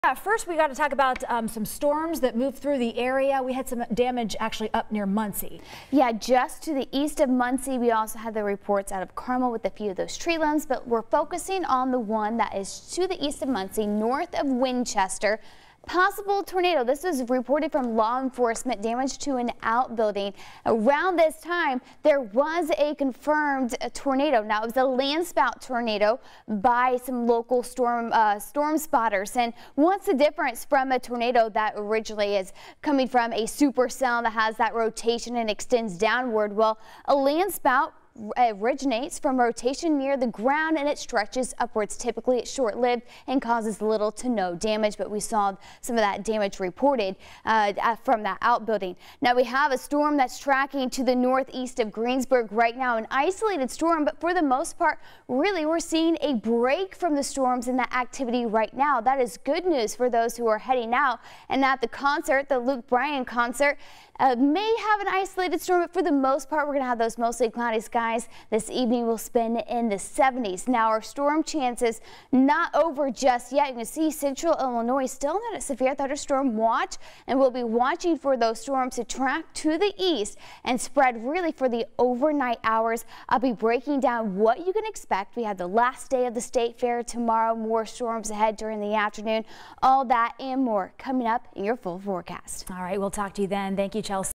Landspout tornado confirmed in Randolph County
A landspout tornado originates from rotation near the ground and stretches upward. It can be short-lived and cause little damage.